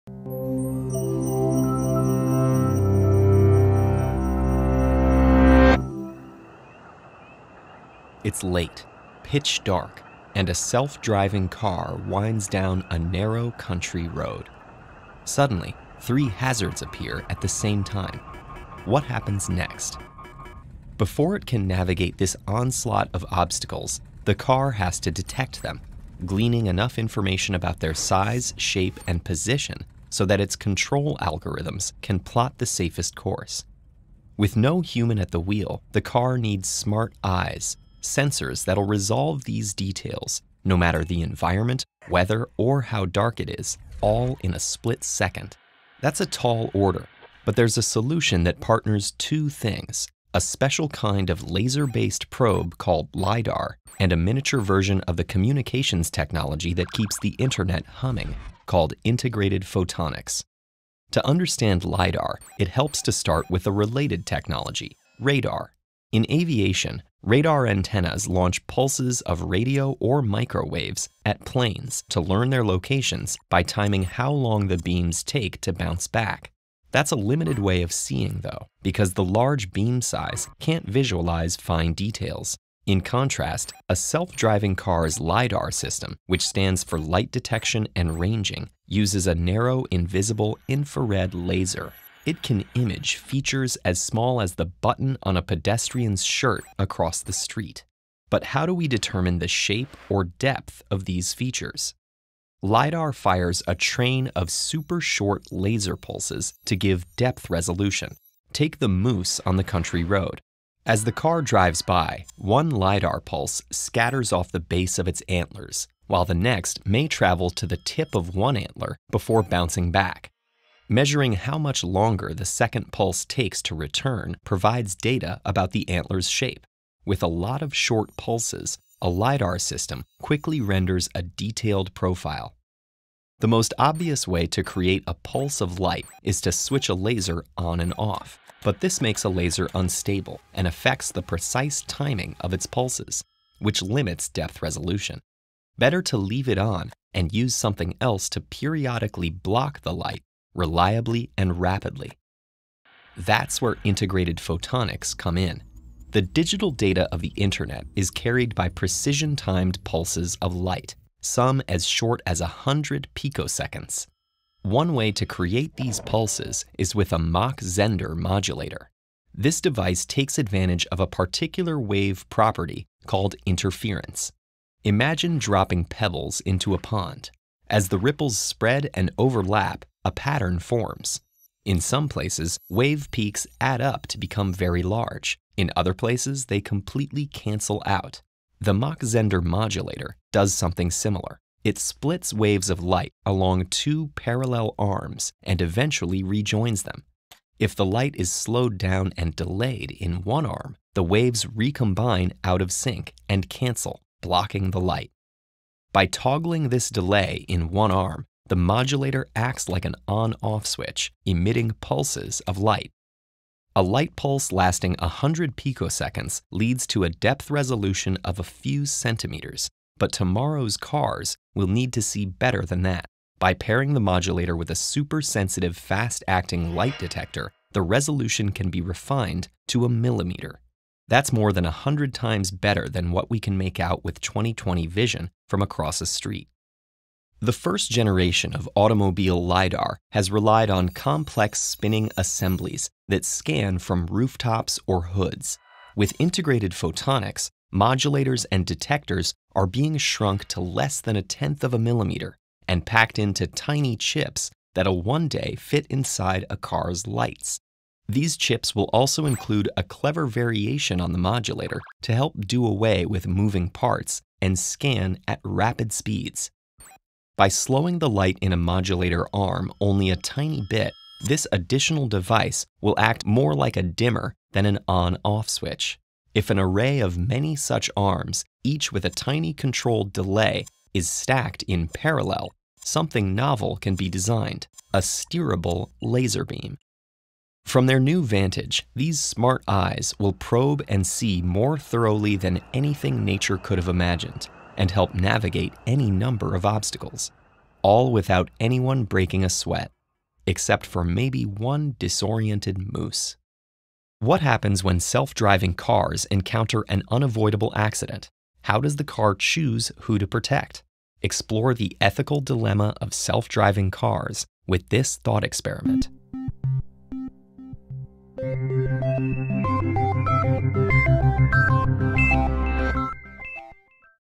[TED Ed - Vietsub] Xe ô tô tự lái nhìn đường như thế nào? - Sajan Saini
Hãy xem LIDAR và các công nghệ quang tử tích hợp giúp xe tự lái điều hướng chướng ngại vật, bất kể môi trường, thời tiết hay ánh sáng.

Ở thời điểm hiện tại, ô tô tự lái vẫn đang là một công nghệ còn mới mẻ, những chiếc xe hơi có khả năng tự lưu thông trên đường là chưa có nhiều. Tuy nhiên, nhìn nhận từ những lợi ích mà nó mang lại, nhiều người cho rằng đây sẽ là tương lai của ngành ô tô.
© The copyright of this video belongs to TED